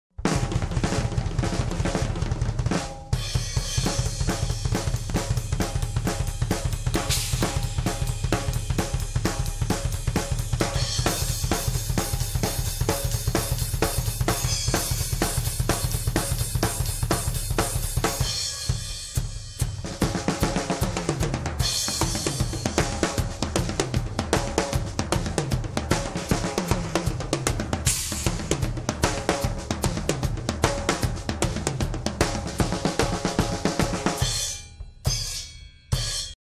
Realistic Rock for Kids: My First Rock & Roll Drum Method, by Carmine Appice
Realistic Rock for Kids is a method by drum legend Carmine Appice. Similar to his award-winning method Realistic Rock, this book shows kids how to play rock 'n' roll drums the quick and easy way. It teaches basics such as note reading, holding the sticks, simple hand/foot coordination exercises, and more. All of the exercises are performed on the accompanying CDs. The discs also include tracks of Carmine rockin' out to some great songs with his band. You can hear how he puts it all together then play along with special remixed drumless versions.